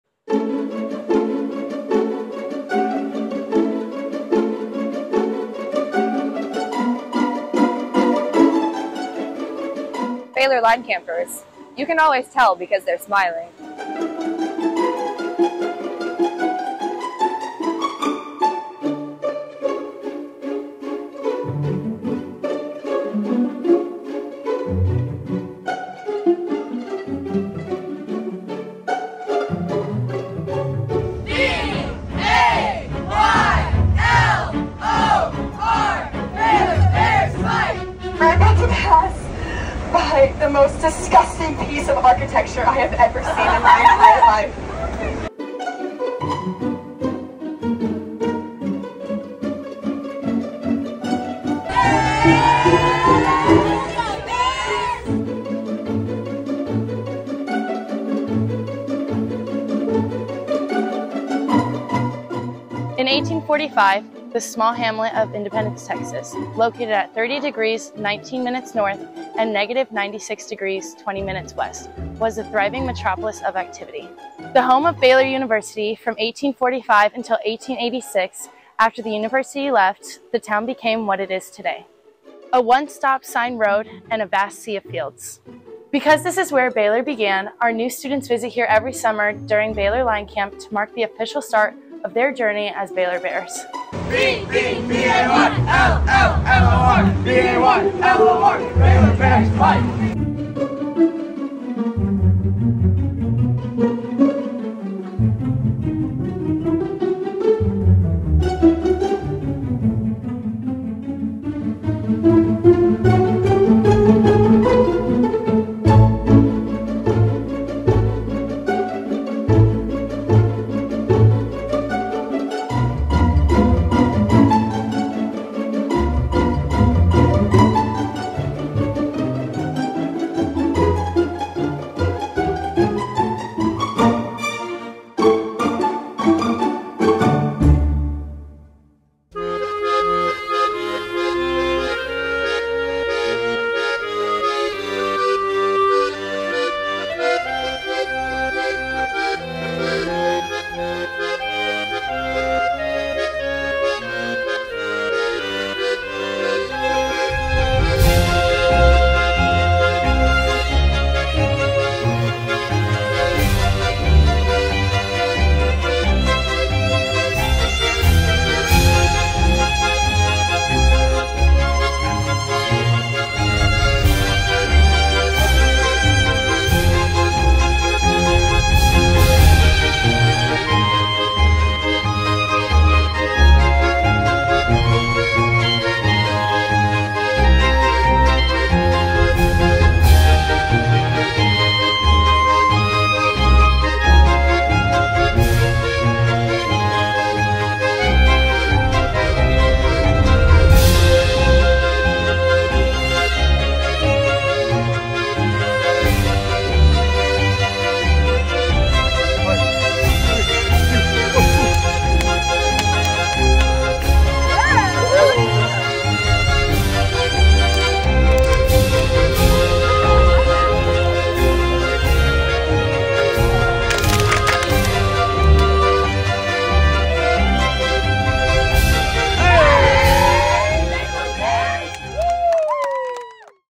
Baylor Line Campers in Independence
My little homage to Wes Anderson...filmed Summer 2023.